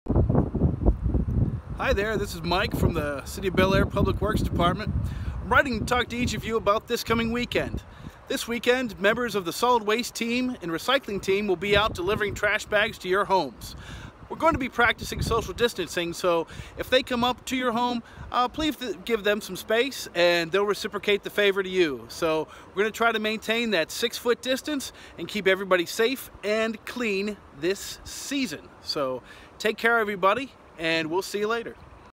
Trash Bag Delivery This Week
A message from Bellaire Public Works Director Michael Leech regarding trash bag delivery.